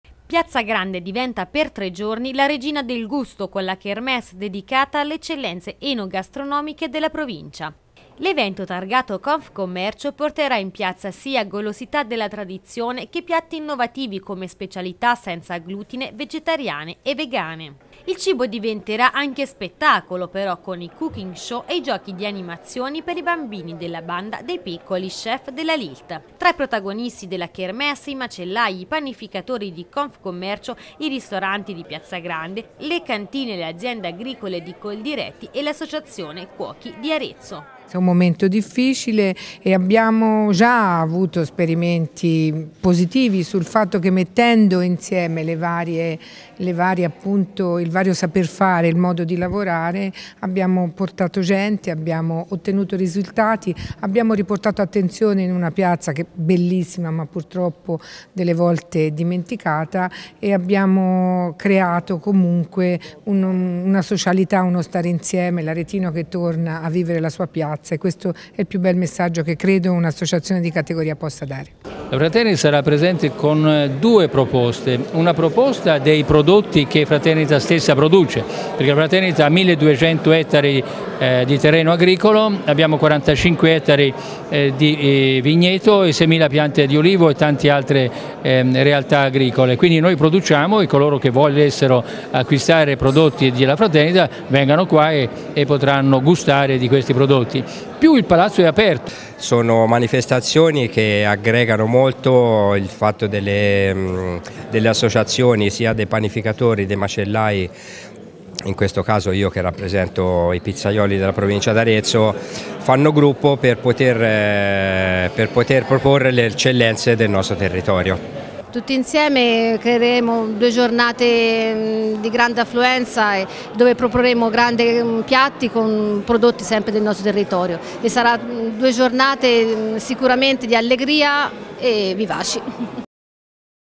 Piazza Grande torna ad essere la piazza del gusto
Sabato 16e  domenica 17 aprile 2016. Evento Confcommercio